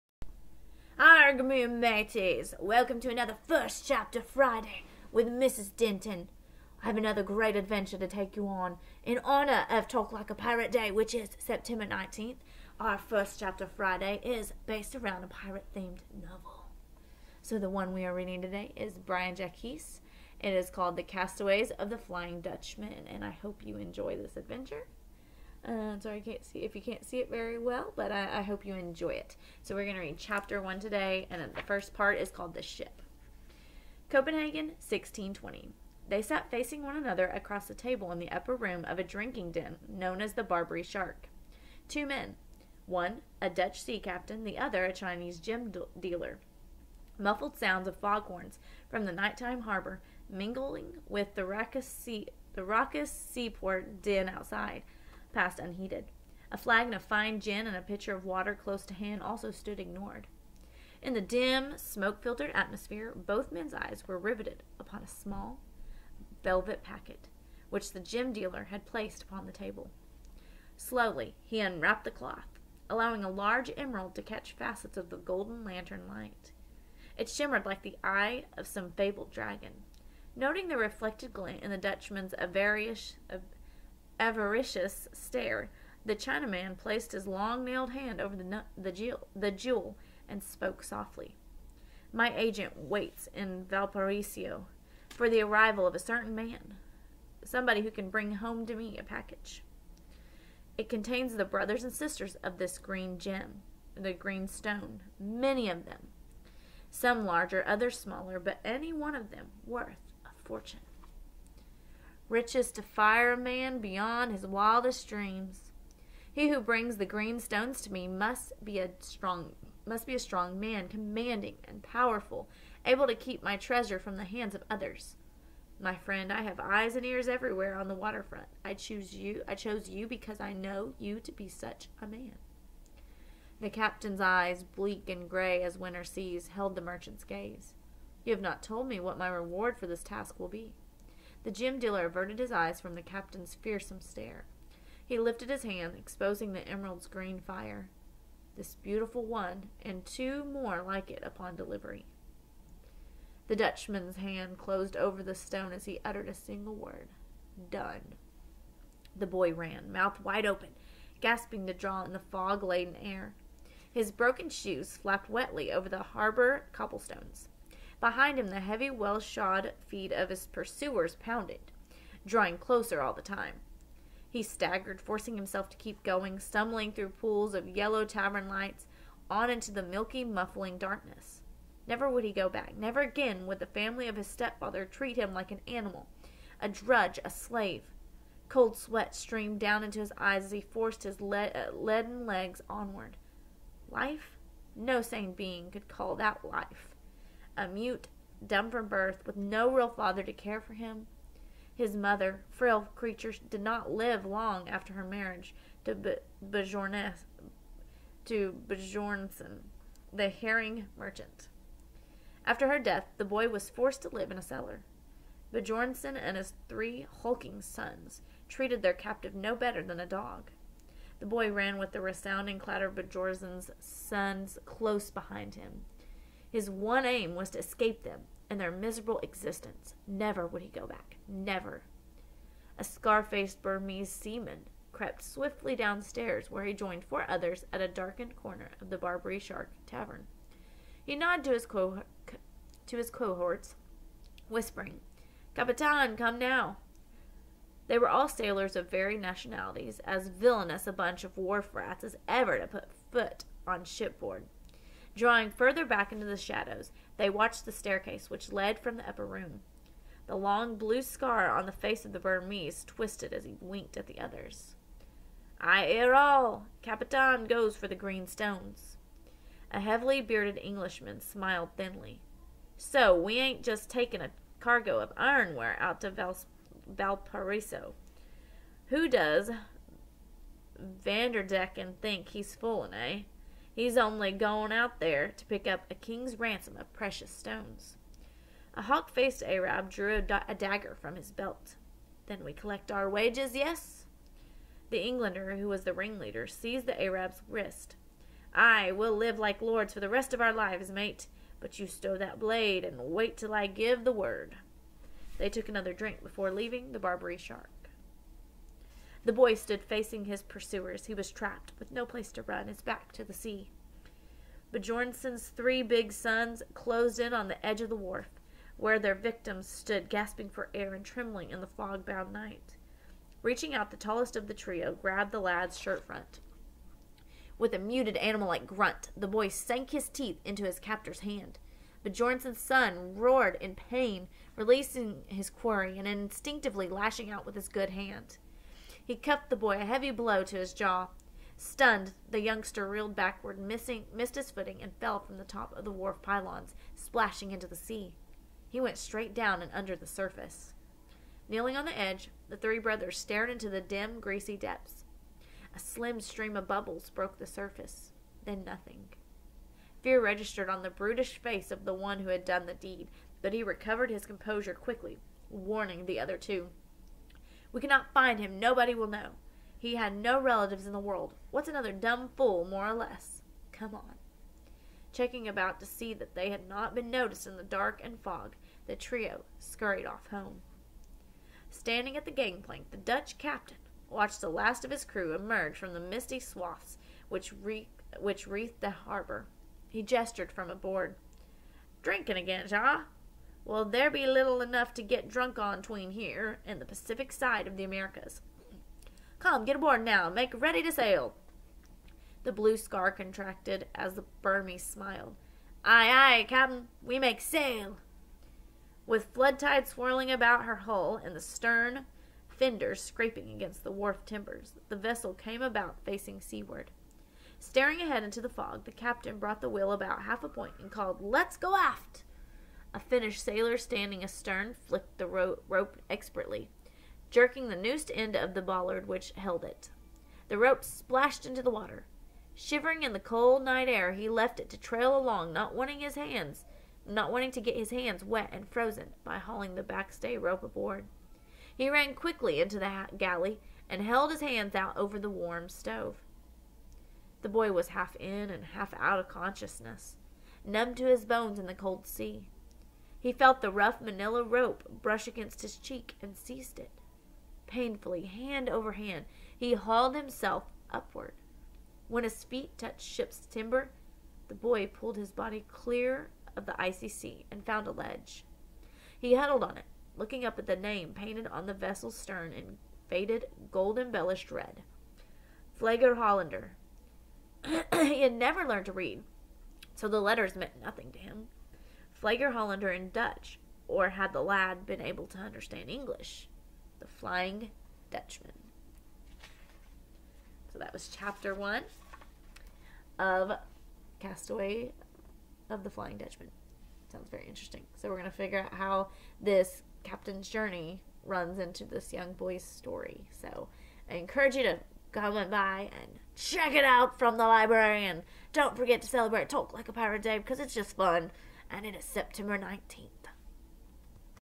First Chapter Friday--Castaways of the Flying Dutchman
In honor of Talk Like a Pirate Day (Sept 19th), enjoy this pirate themed novel by Brian Jacques!